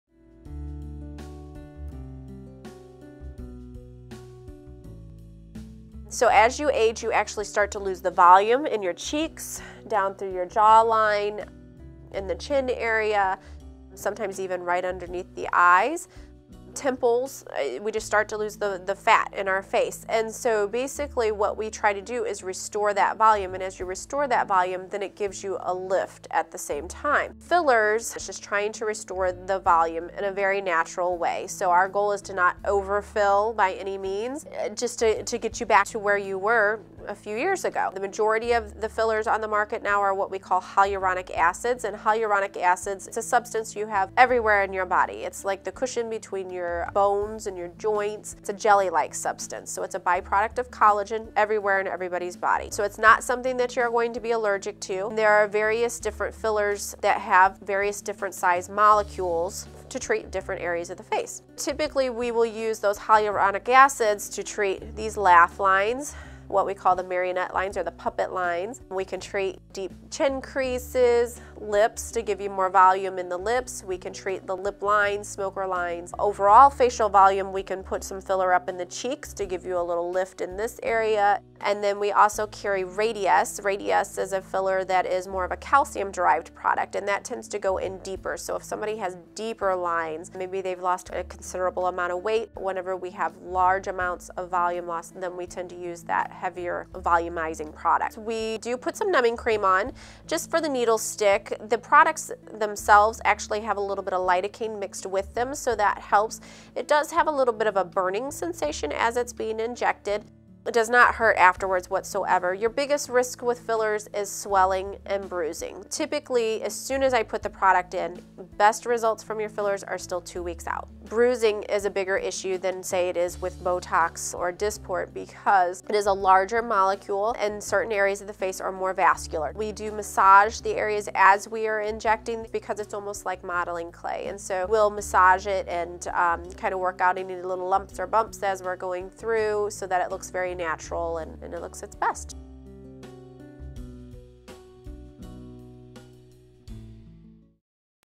Wrinkle Reduction With Non-Surgical Filler Treatments
Dermal facial fillers are used for lines caused by repetitive motion of a muscle. This repetitive use of facial muscles results in dynamic wrinkles, wrinkles that become static and more evident over time as we age. We offer several filler solutions to smoothing out and plumping these types of facial wrinkles. These dermal fillers work by boosting your skin's natural production of hyaluronic acid (HA) to add volume and diminish wrinkles, resulting in a revitalized and more youthful appearance. Read more about each individual filler option we have on our dermal fillers page on our